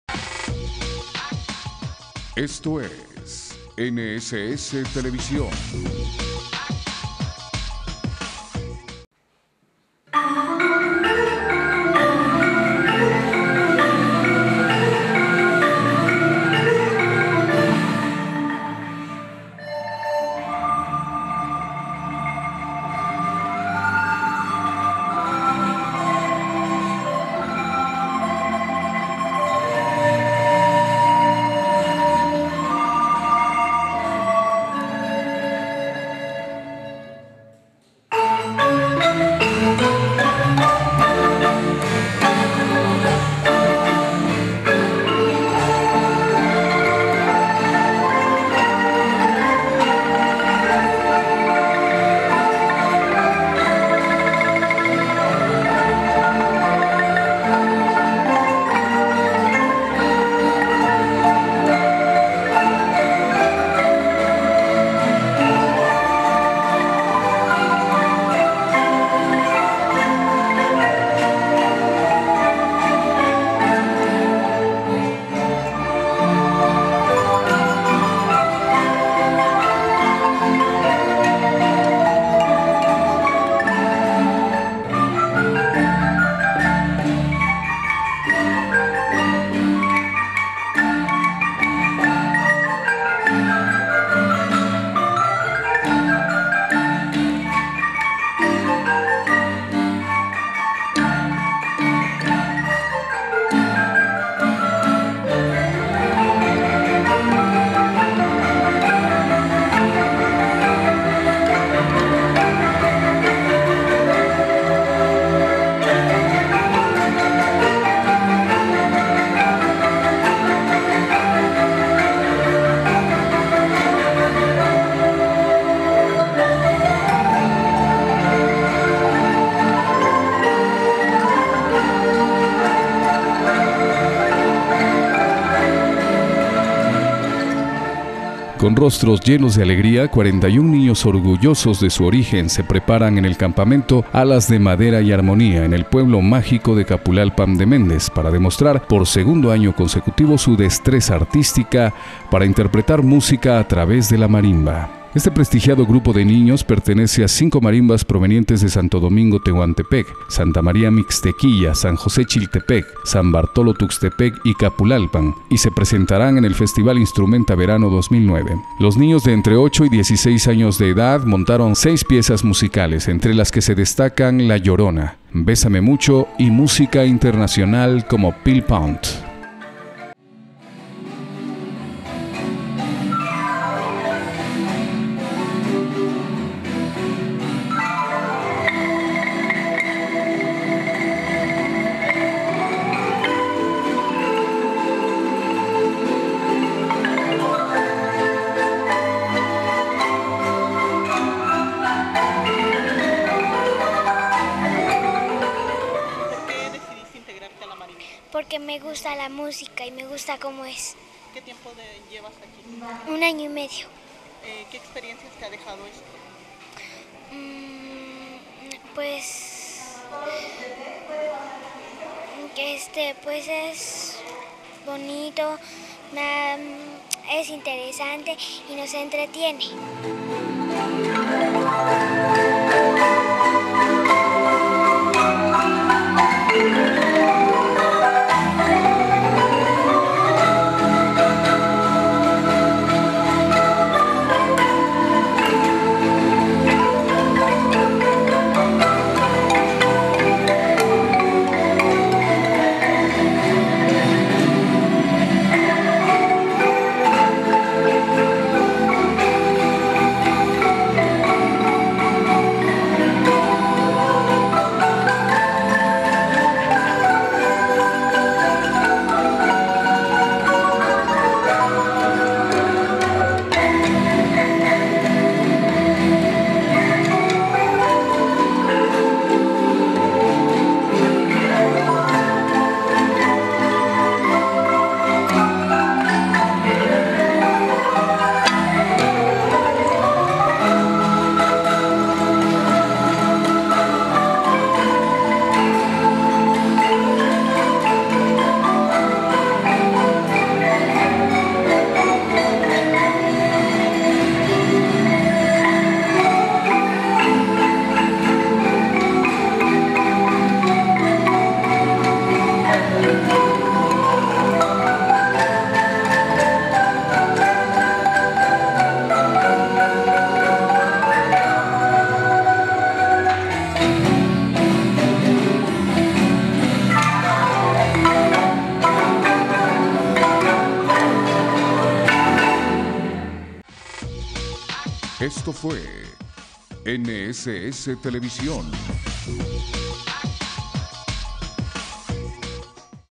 Marimba: Alas de madera y armonía
Reportaje de los niños del campamento Marimba: Alas de madera y armonía ensaya en el pueblo mágico de Capulalpam de Méndez, en la sierra de Oaxaca, y se preparan para participar en Instrumenta Verano 2009